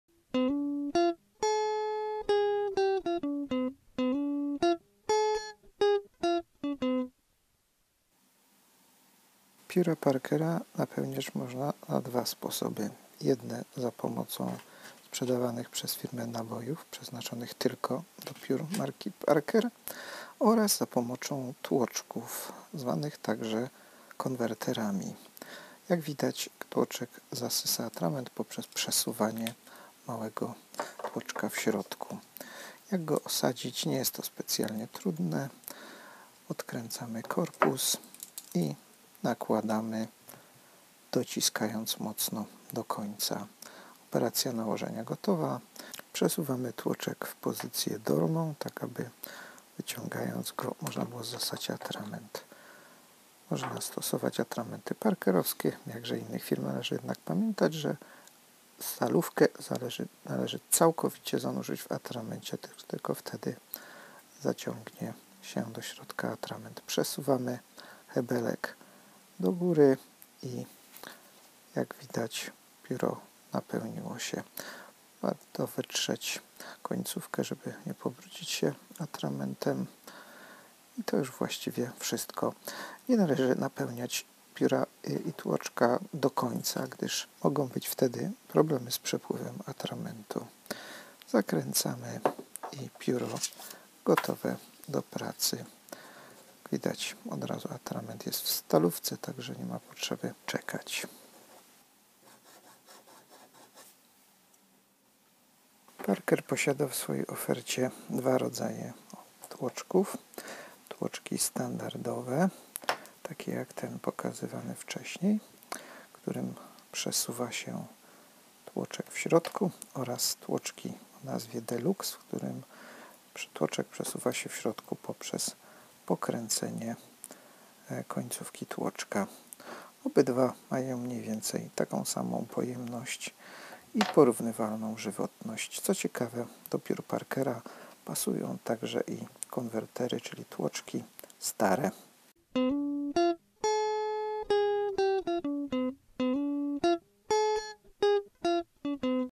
Pióra wieczne Parker - napełnia pióra za pomocą tłoczków / konwerterów
Twoje Pióro prezentuje film "Pióra wieczne Parker - napełnia pióra za pomocą tłoczków / konwerterów".